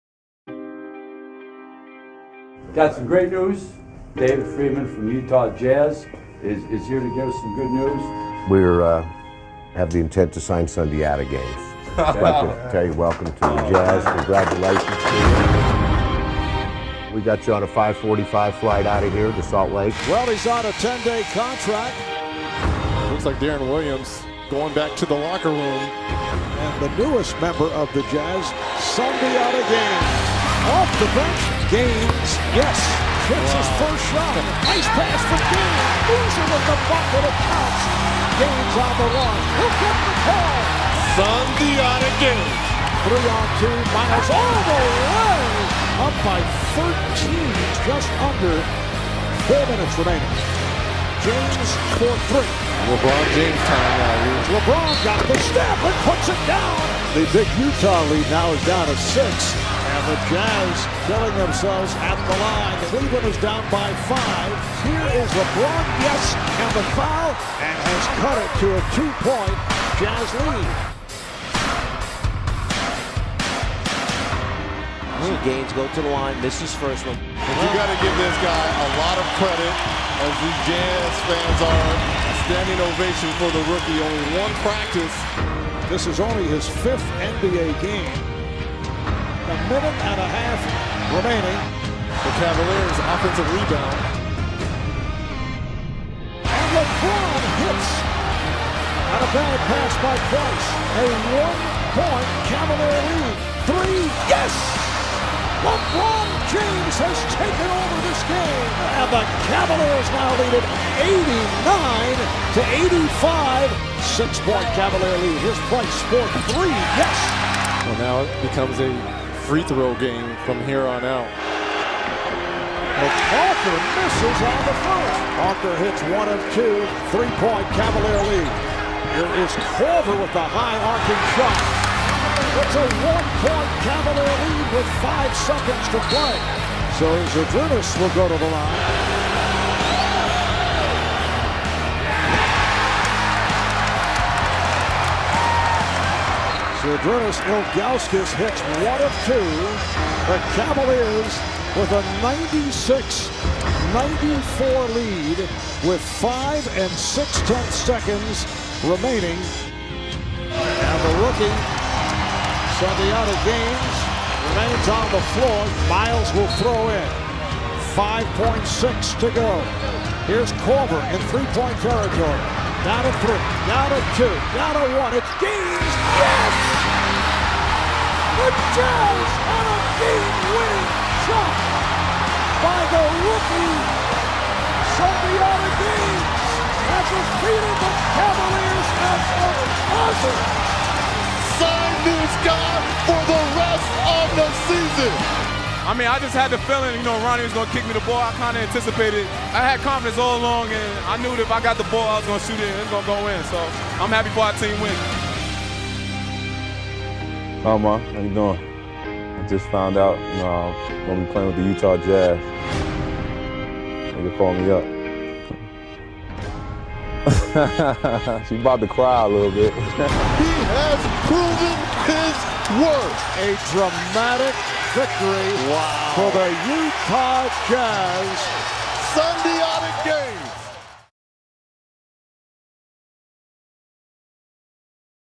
A Finish To Remember - Jazz Cavaliers Edition
"As he stood on the scorer's table Thursday night in the middle of a delirious EnergySolutions Arena, having hit a shot so spectacular it eclipsed LeBron James' starburst, Sundiata Gaines was evidence that NBA dreams do come true."

Ross Siler
The Salt Lake Tribune

Thanks to TNT for the footage!